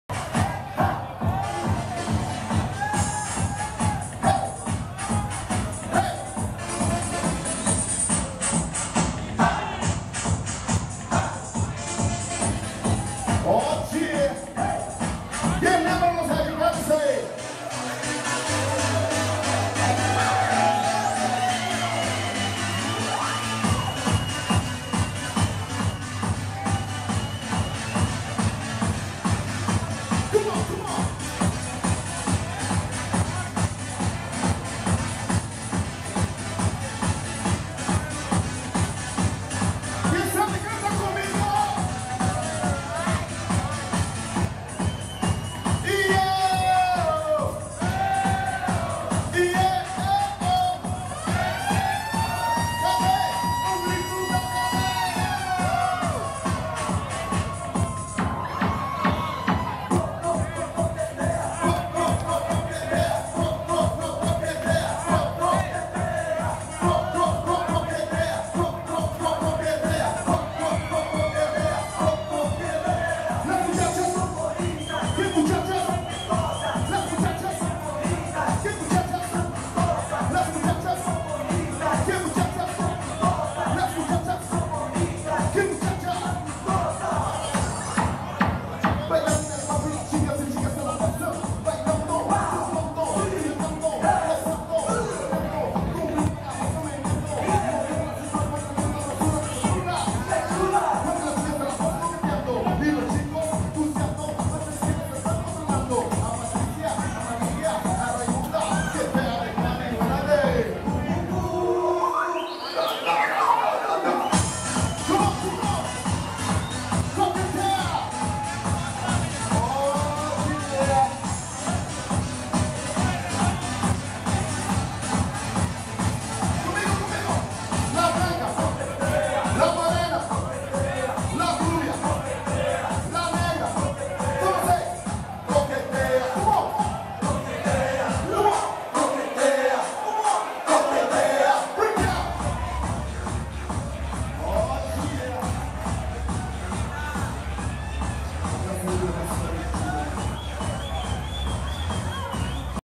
``Richard Four´´ DETONANDO NA SPECTRON DISCO SHOWWWW
MANAUS 11/04/2015 SHOW COM MEU AMIGO Richard Four UMA APRESENTAÇÃO SHOWWWWWWWWWWW